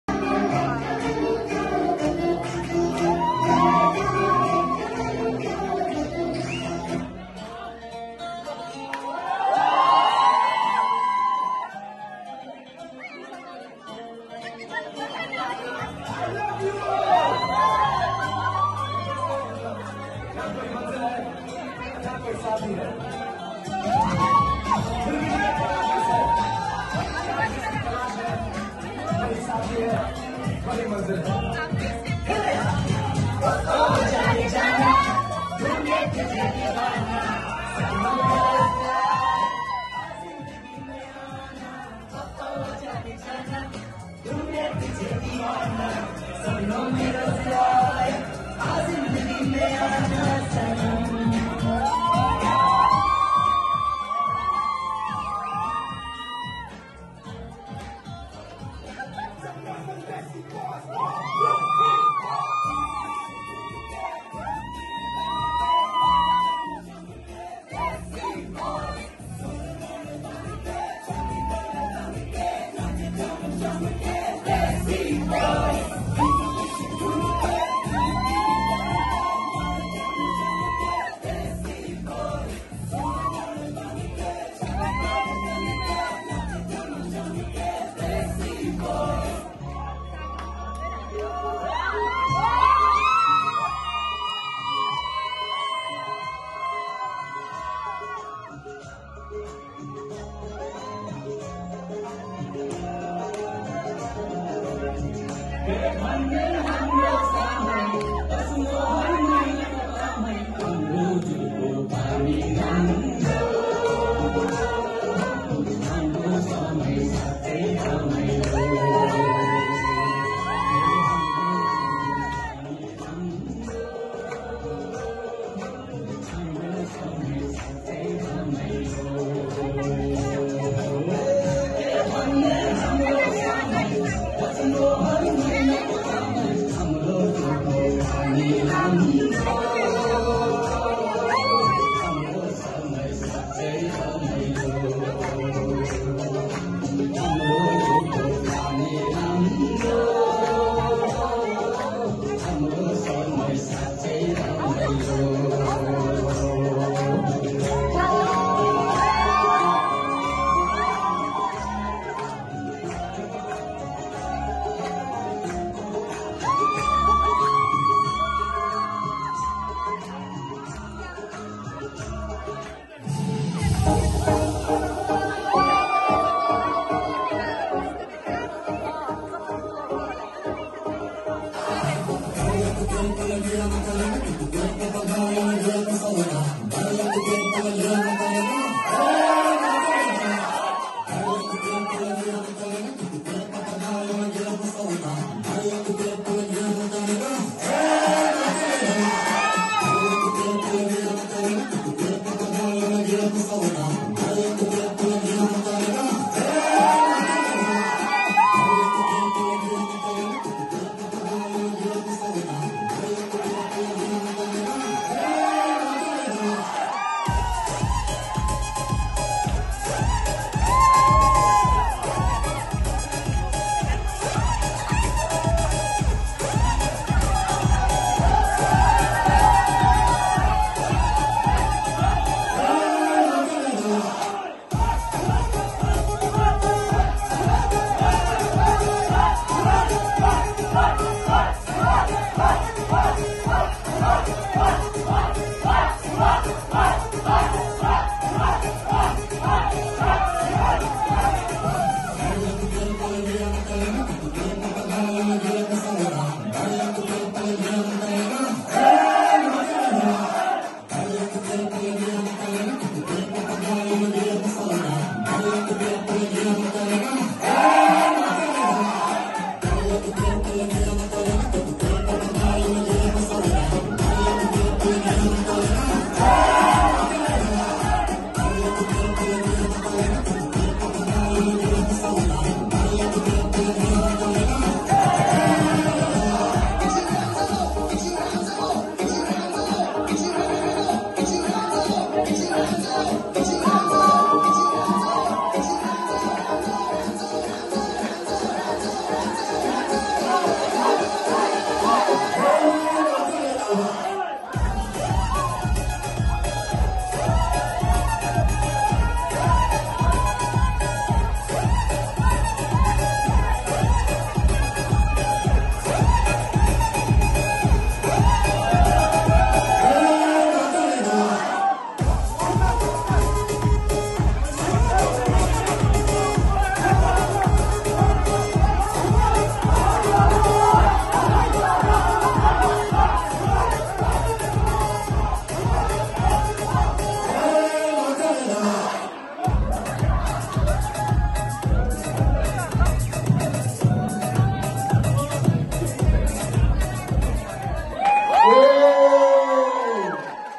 Groom Mates dance to surprise bride and groom@ Nepali wedding Canberra Australia
Can’t believe these boys practiced just for 2hrs. They did really well 🙌🙌